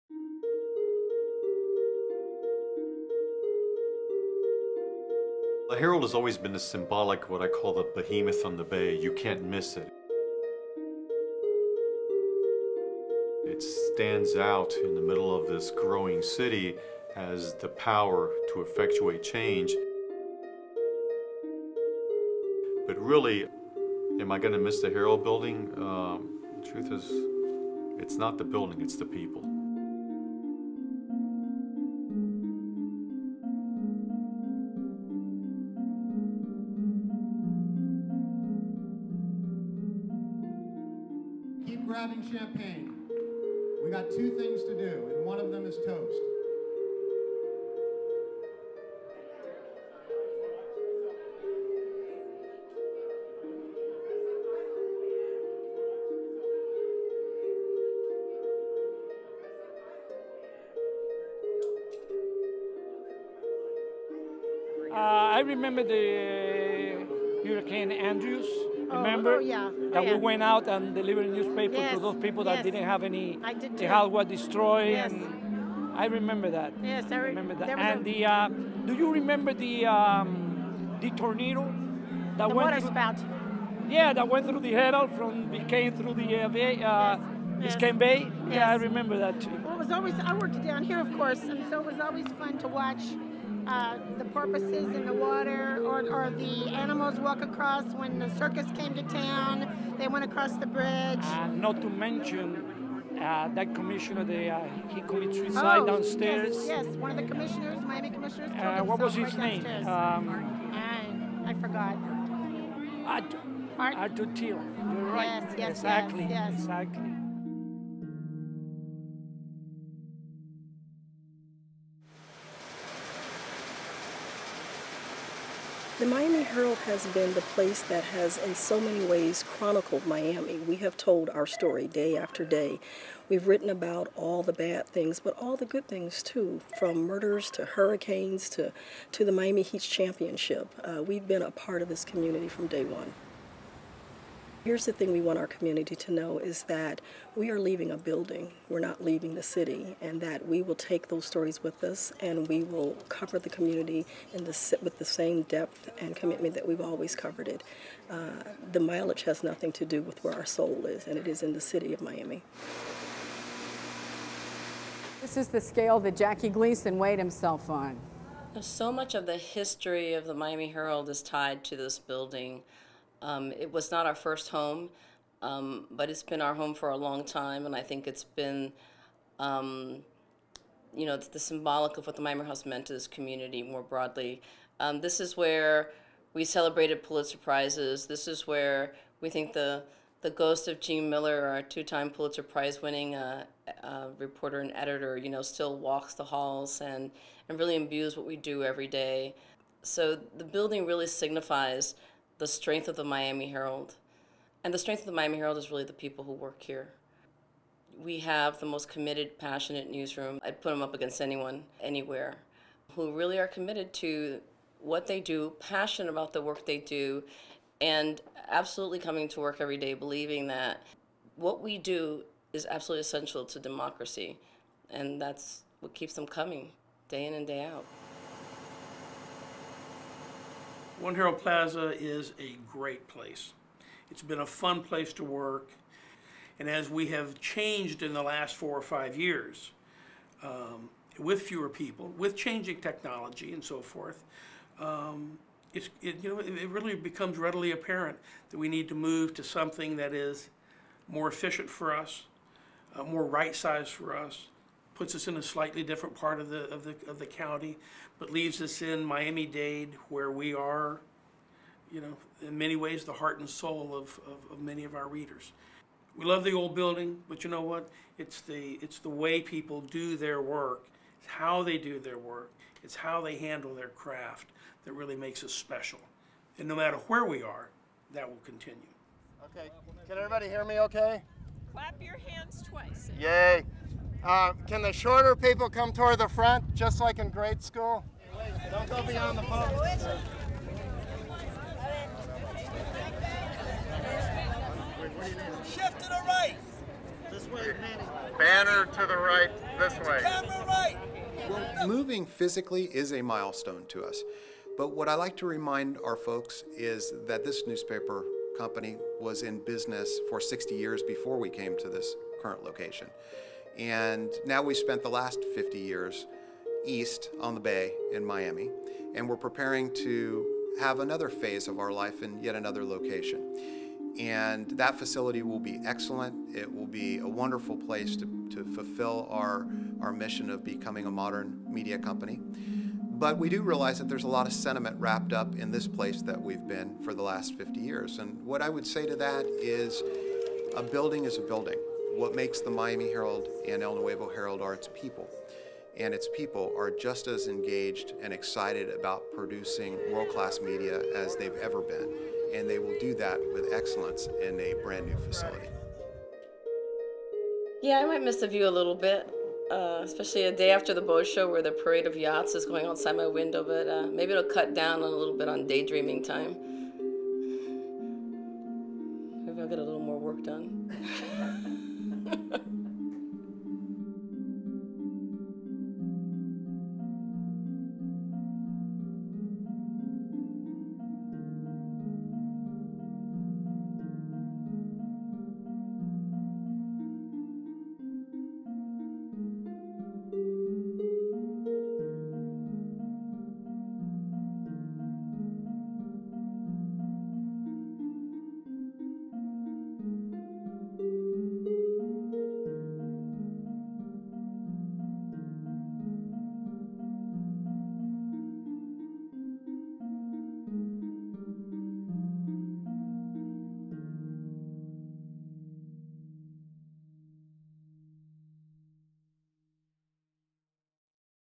The Miami Herald's big move
The Miami Herald and El Nuevo Herald say goodbye to their downtown headquarters and prepare for the move to Doral. Jose A. Iglesias/El Nuevo Herald Staff